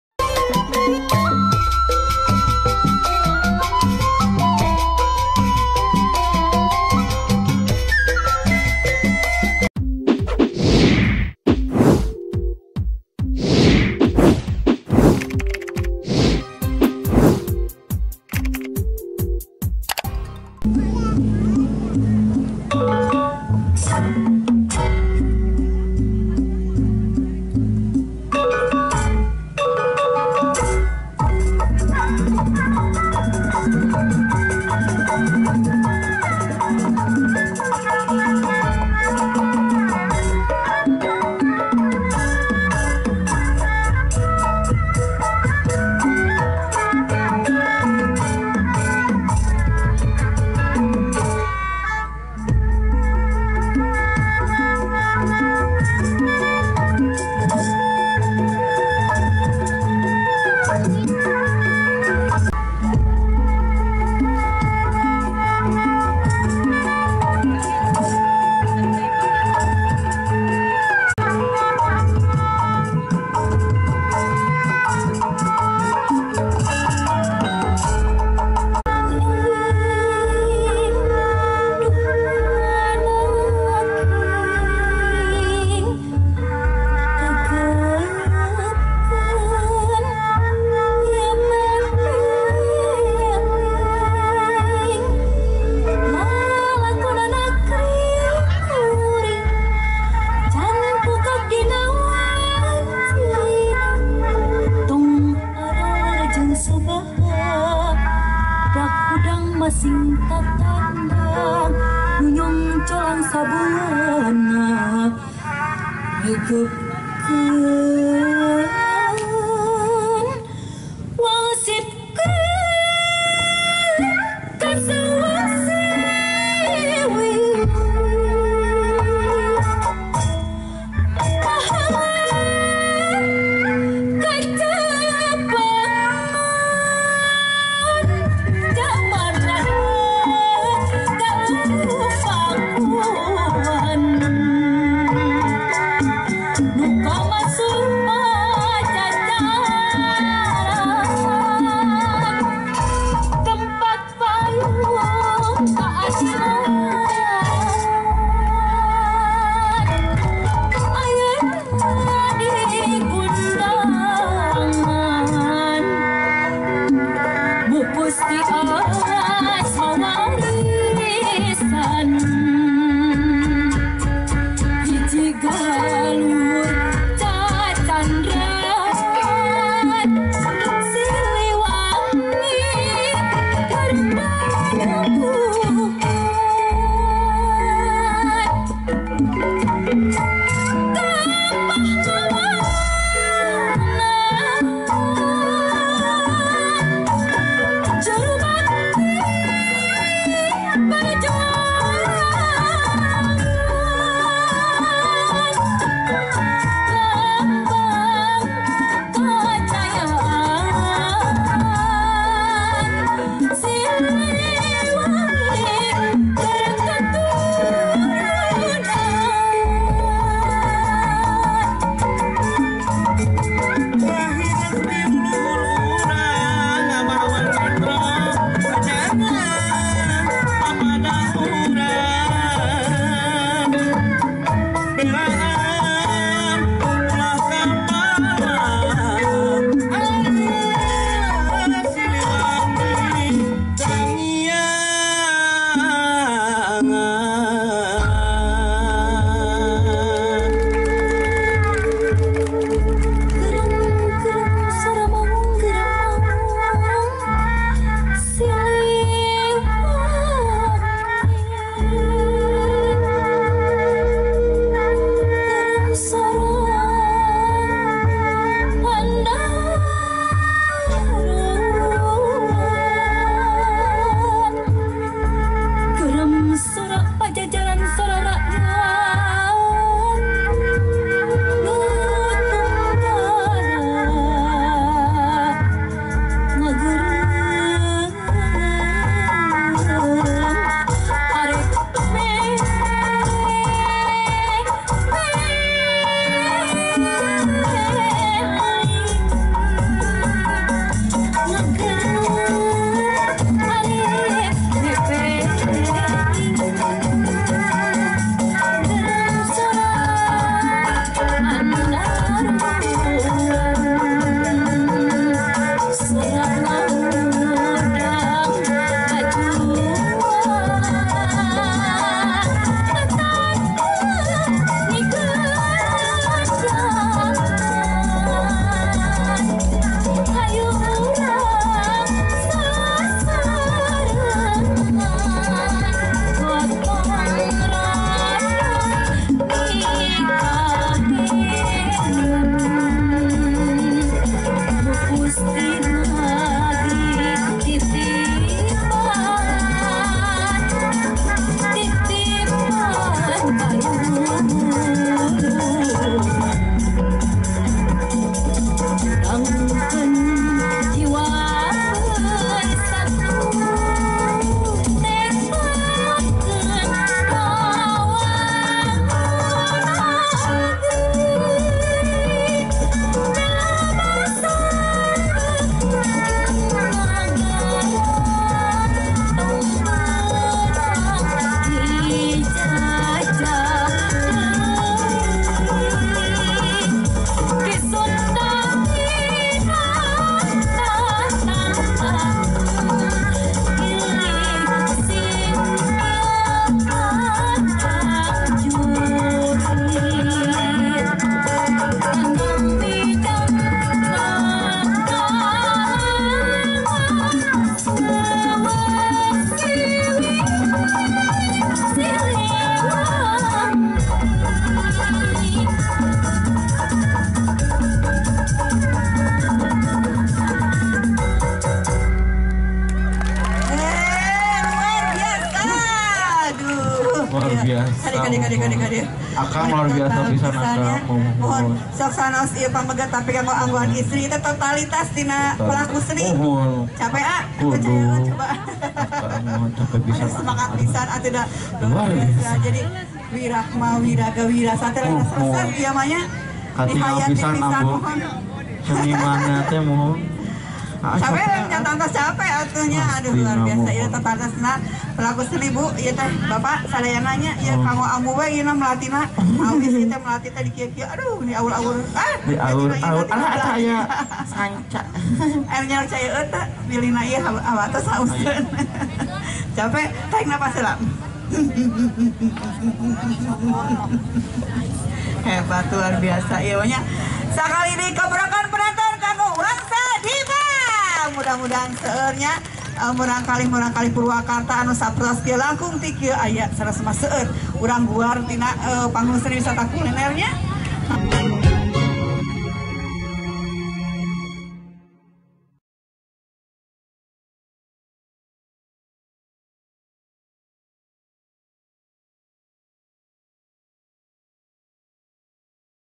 Kereen Pelatih Jaipong Wangsa Dipa Naik Panggung Cikopo Bungursari Purwakarta
Kereeeen Seorang Pria Pelatih Jaipong Dasa Sima Naik Panggung